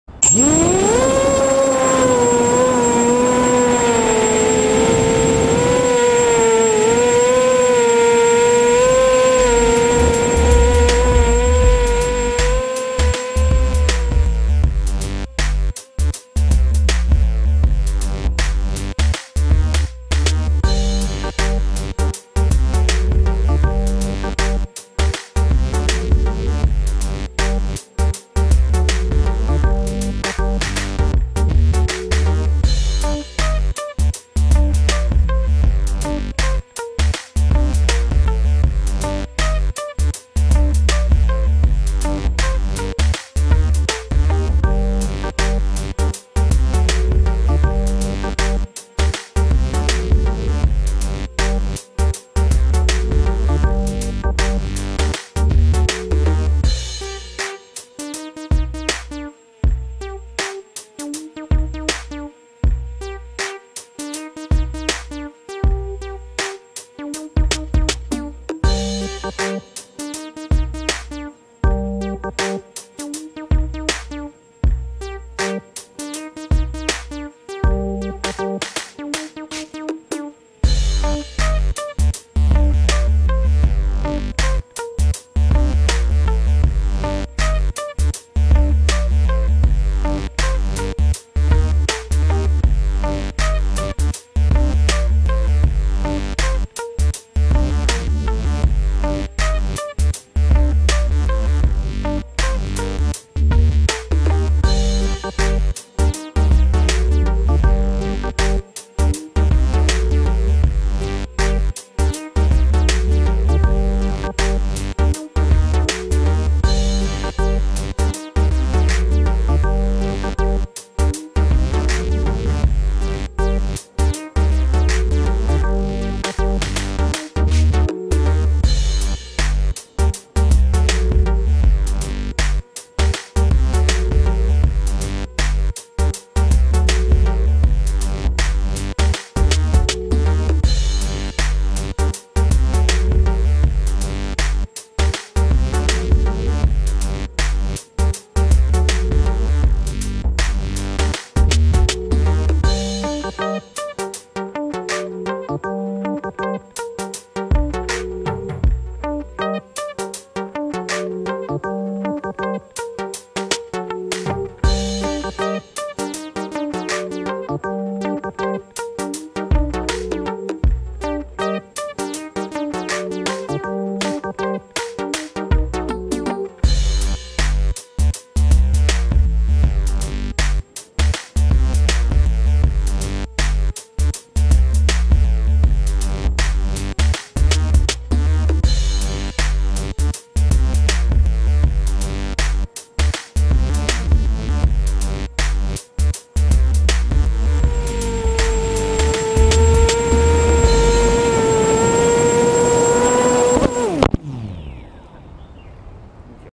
weekend flying 2015-11-22. Featuring illuway
Another weekend with my best friend.  This should be the last time he fly with this 330 sized Multirotor.